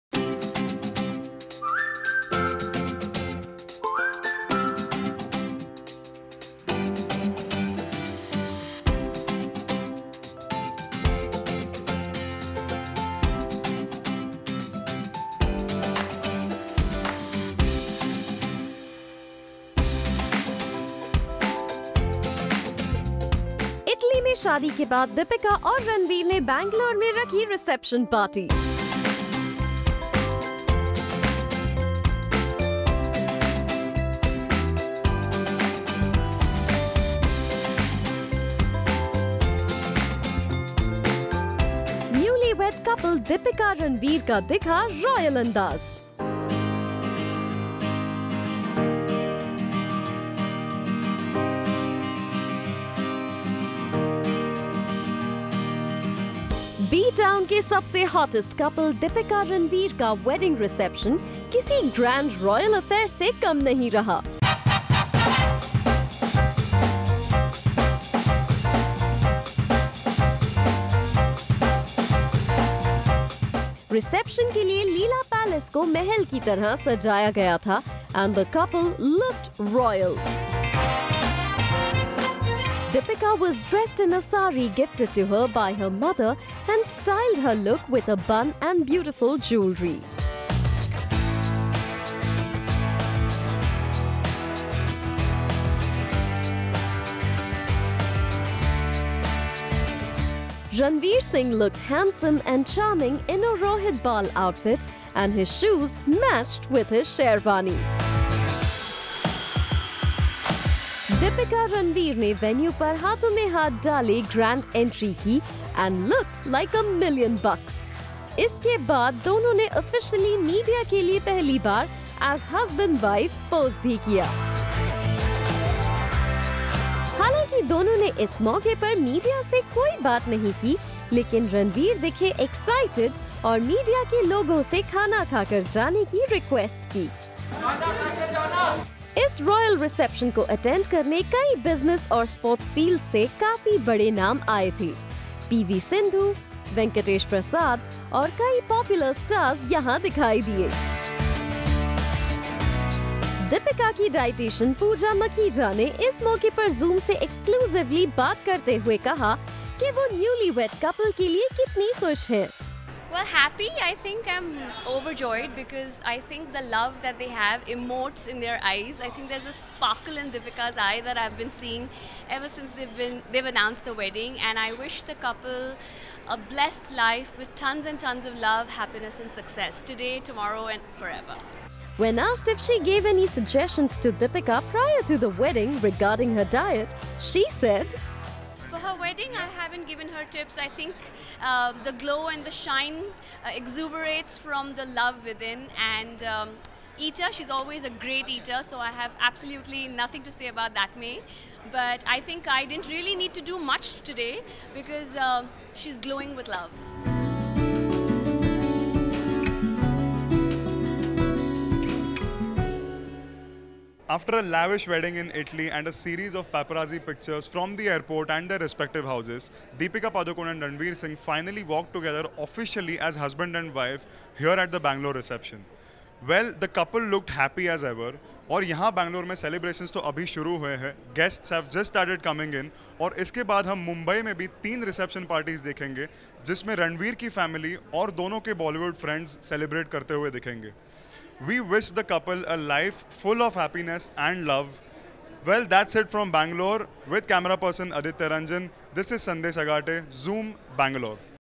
Deepika Padukone's dietitian reveals secret behind the bride's extra glow
Deepika Padukone and Ranveer Singh’s lavish wedding has been the talk of the town since long time and now the newlywed is all set to host their second reception which is to take place in Mumbai. In an exclusive chat with Zoom at couple’s Bengaluru reception, Deepika’s dietician Pooja Makhija spilled the beans on her bridal glow. She said, "I think the love that they have emotes in their eyes, I think there is sparkle in Deepika's eye that I have been seeing ever since they (Ranveer and Deepika) have announced their wedding. I wish the couple a bless life with tons and tons of love, happiness and success today tomorrow and forever." When asked if she gave any suggestion to Deepika prior to the wedding regarding her diet Pooja said, "For her wedding, I haven't given her tips. I think the glow and the shine exuberated from the love within and she is always a great eater, so I have nothing to say about that, but I think I don't really needed to do much today because she is glowing with love." Watch the video for more details.